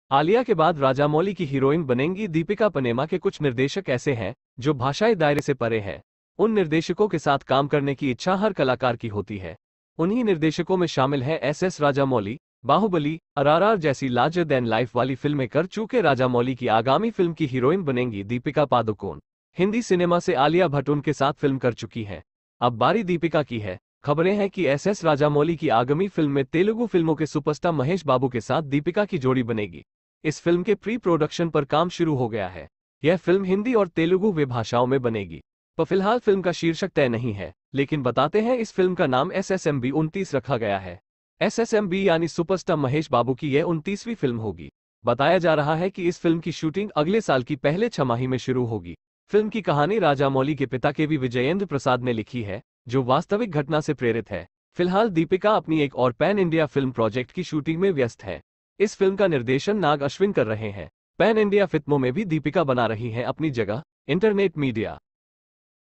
आलिया के बाद राजामौली की हीरोइन बनेंगी दीपिकाप्नेमा के कुछ निर्देशक ऐसे हैं, जो भाषाईदायरे से परे है
आलिया के बाद राजामौली की हीरोइन बनेंगी दीपिका
प्नेमा के कुछ निर्देशक ऐसे हैं, जो भाषाई
दायरे से परे हैं। उन निर्देशकों के साथ
काम करने की इच्छा हर कलाकार की होती है।
उन्हीं निर्देशकों में शामिल हैं एसएस राजामौली।
बाहुबली, अरआरआर जैसी लार्जर दैन लाइफ
वाली फिल्में कर चूके राजामौली की आगामी
फिल्म की हीरोइन बनेंगी दीपिका पादुकोण।
हिंदी सिनेमा से आलिया भट्ट उनके साथ
फिल्म कर चुकी हैं। अब बारी दीपिका की है।
खबरें हैं कि एसएस राजामौली की आगमी
फिल्म में तेलुगु फिल्मों के सुपरस्टार महेश
बाबू के साथ दीपिका की जोड़ी बनेगी। इस
फिल्म के प्री-प्रोडक्शन पर काम शुरू हो गया
है। यह फिल्म हिंदी और तेलुगु वे भाषाओं
में बनेगी। पफिलहाल फिल्म का शीर्षक तय
नहीं है, लेकिन बताते हैं इस फिल्म का नाम
एसएसएमबी 29 रखा गया है। एसएसएमबी
यानी सुपरस्टार महेश बाबू की यह 29वीं
फिल्म होगी। बताया जा रहा है कि इस फिल्म
की शूटिंग अगले साल की पहले छमाही में
शुरू होगी। फिल्म की कहानी राजामौली के
पिता केवी विजयेंद्र प्रसाद ने लिखी है, जो
वास्तविक घटना से प्रेरित है। फिलहाल दीपिका
अपनी एक और पैन इंडिया फिल्म प्रोजेक्ट की
शूटिंग में व्यस्त हैं। इस फिल्म का निर्देशन नाग
अश्विन कर रहे हैं।
पैन इंडिया फित्मों में भी दीपिका बना रही हैं अपनी जगह।